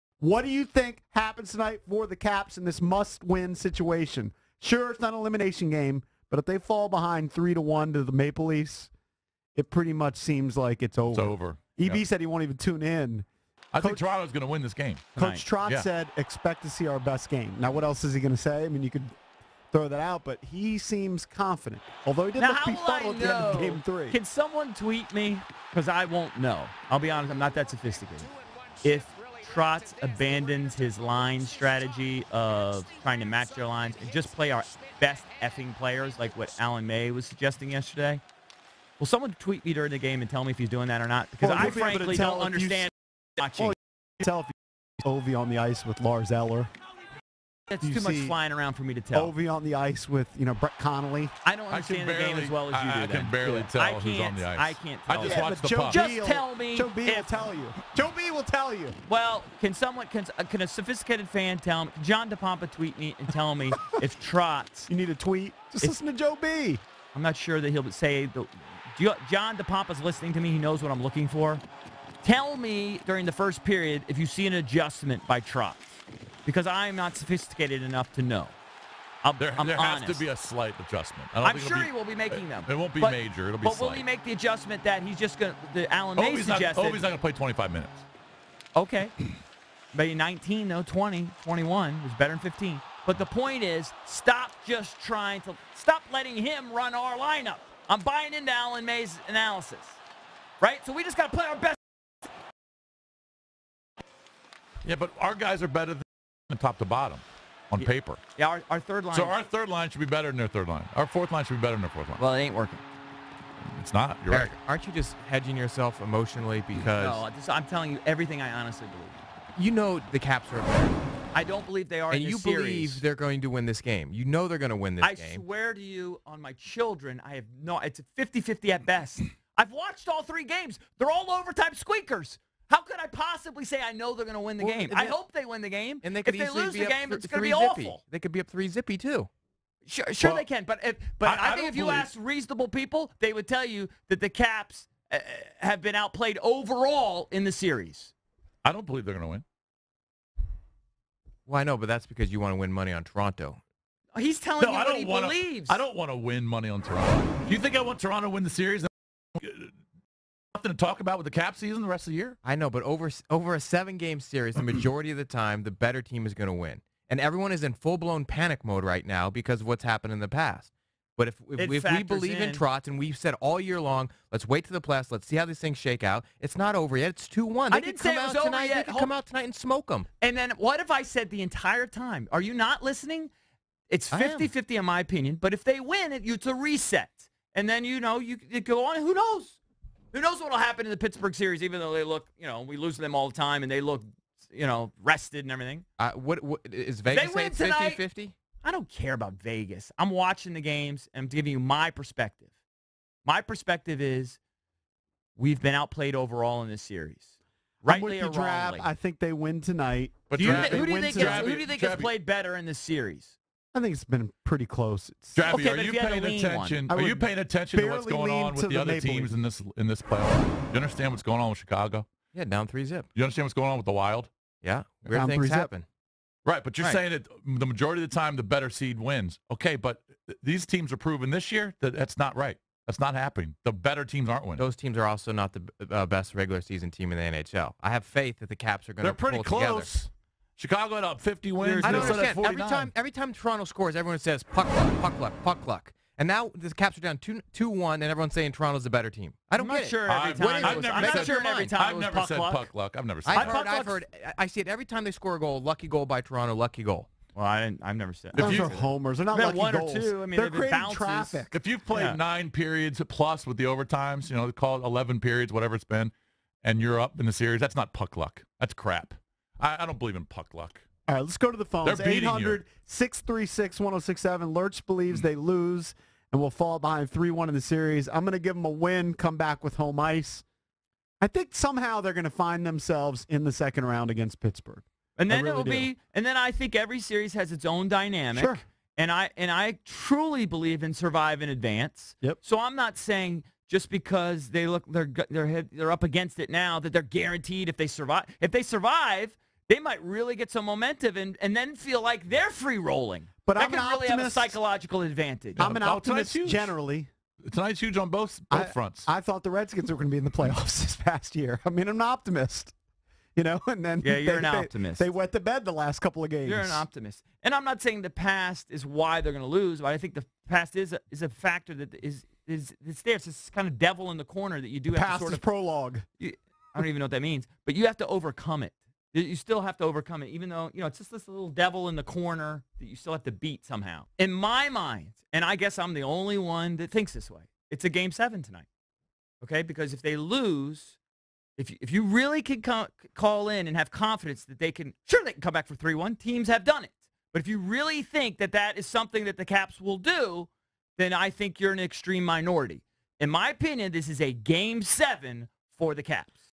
Puck Luck?
Do the Washington Capitals have a response tonight for the Maple Leafs?